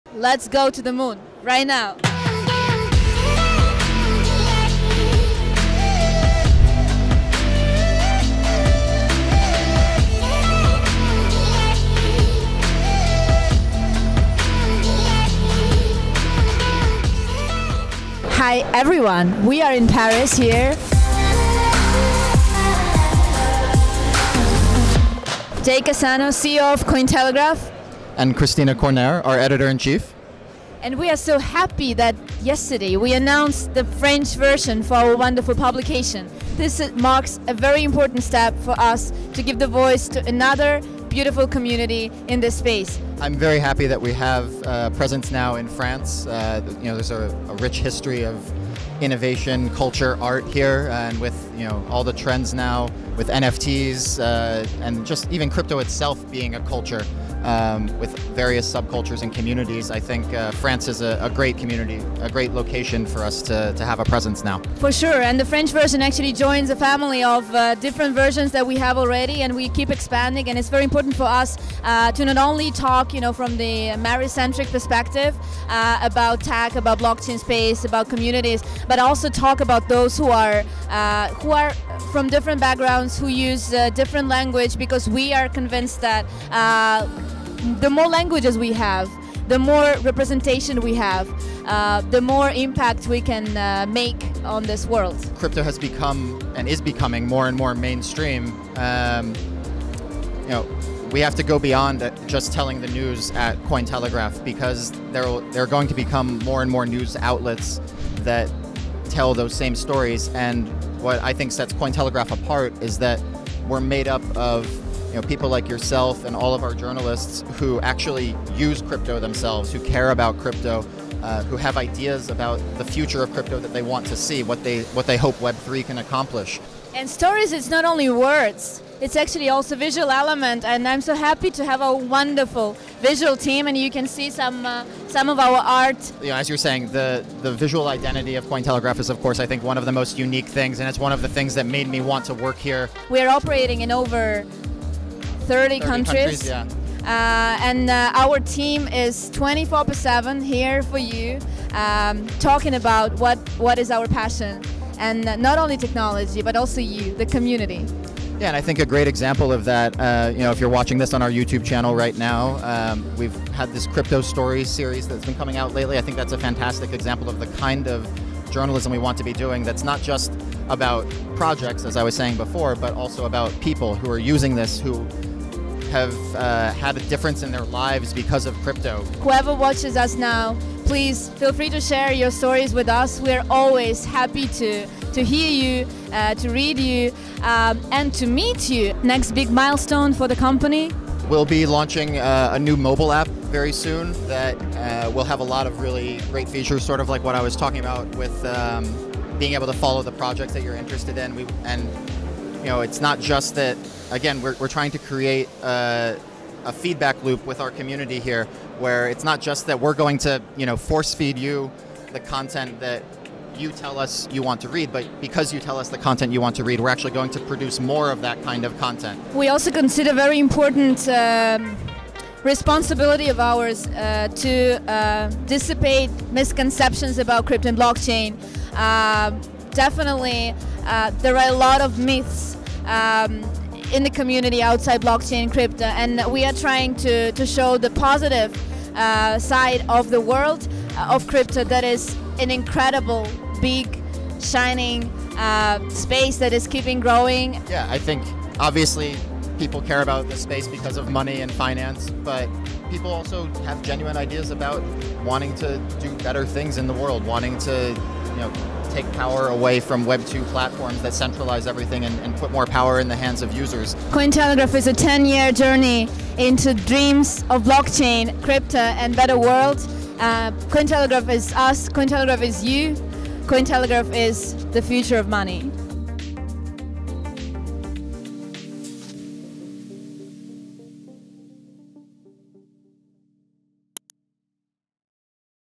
A message to you from Cointelegraph’s CEO and editor-in-chief
Cointelegraph CEO Jay Cassano and editor-in-chief Kristina Lucrezia Cornèr talk about what sets Cointelegraph apart, the company’s goals, new milestones and its new French regional version.